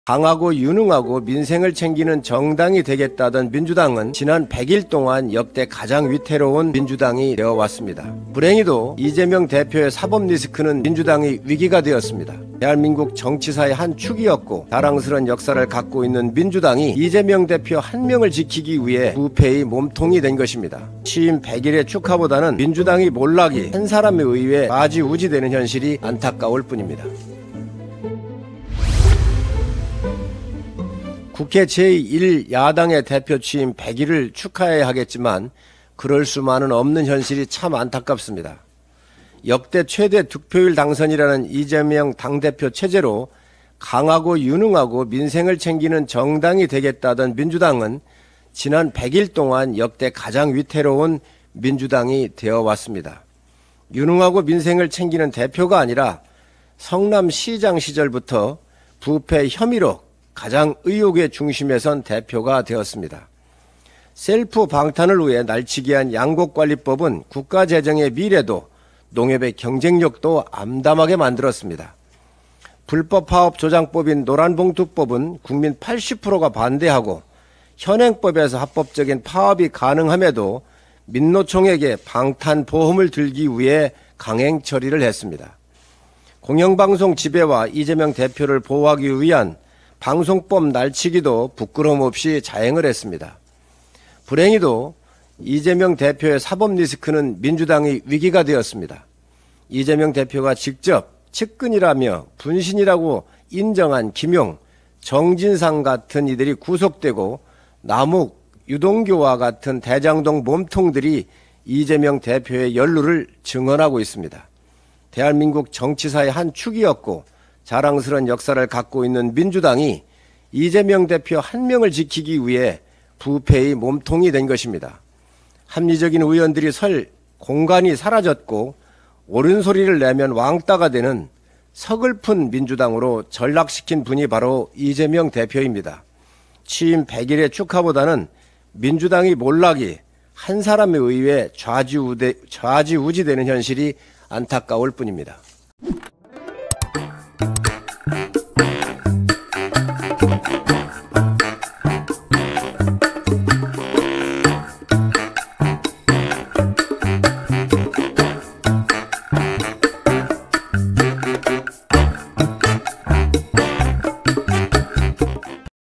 성일종 "민주당, 이재명 한 명을 지키기 위해 부패의 몸통이 돼"
성일종 국민의힘 정책위의장이 22년 12월 5일 서울 영등포구 여의도 국회에서 열린 국민의힘 비상대책위원회의에서 발언하고 있다.